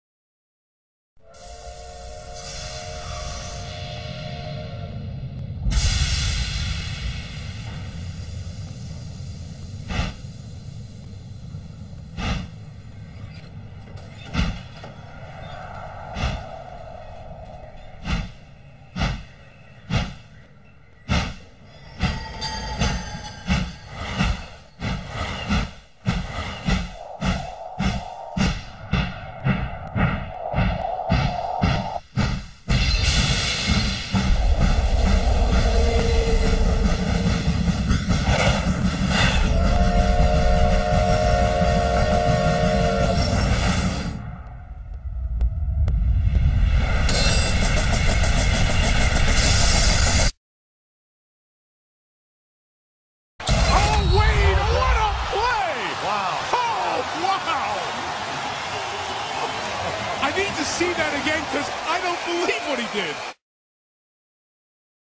The Unstoppable Dwyane Wade Train (Jordan Ad Remix)
Hop on board the D-Wade Train! Dwyane Wade like you've never seen him!

This video is dedicated to fans of the NBA truly happy the lockout is finally ending ;)